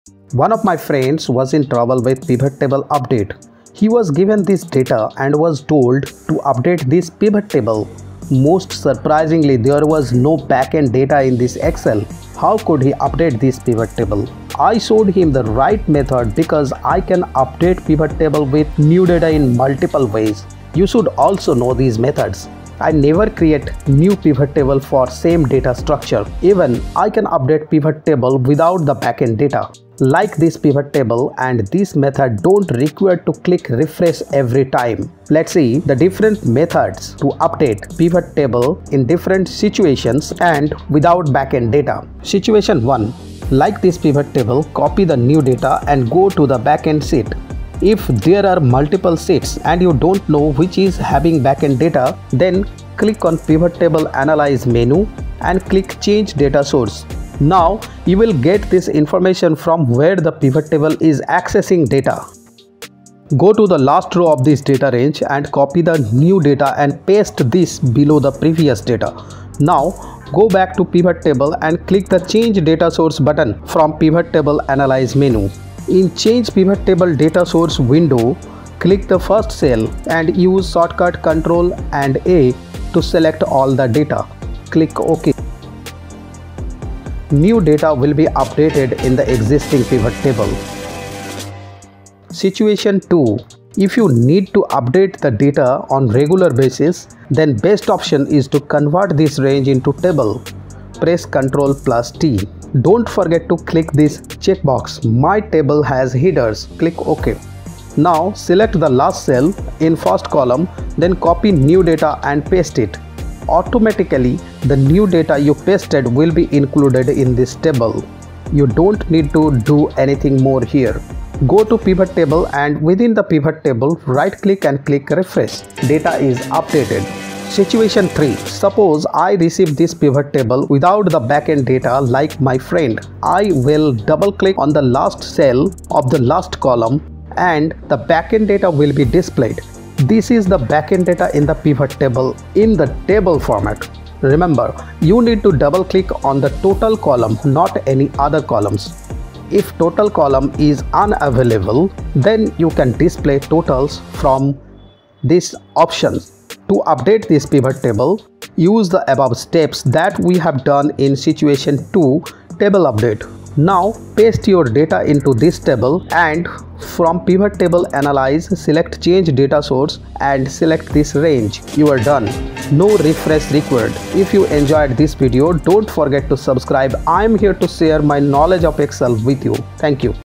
Update Data In Existing Pivot Table In Multiple Ways Even Without Backend Data
Once a pivot table is created sometimes, we need to update this pivot table with new data periodically. In this video I have discussed how to update data in existing Excel Pivot Table instead of creating new Pivot tables everyday. I have discussed few methods to update existing pivot table with data, static and dynamic.
Also, I have discussed how to updeate pivottable without backend data.